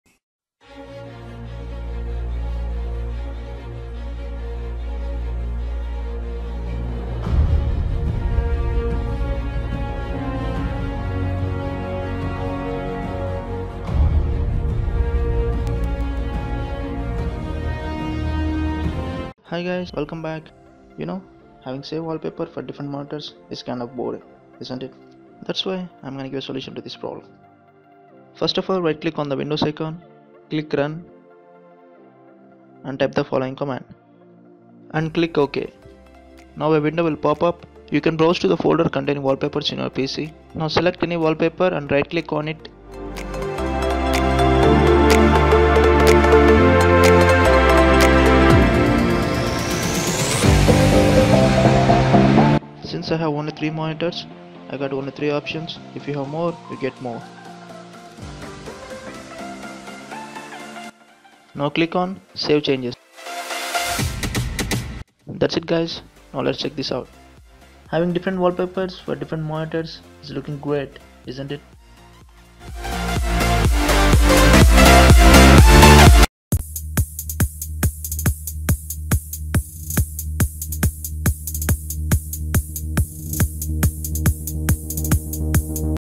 [100% works] How to set different Wallpapers for different Monitors on Windwos 10 \\ 2017
if u have dual or triple monitor setup and u want to set seperate wallpaper for each one. then here is the solution ...

 command :
" control /name Microsoft.Personalization /page pageWallpaper "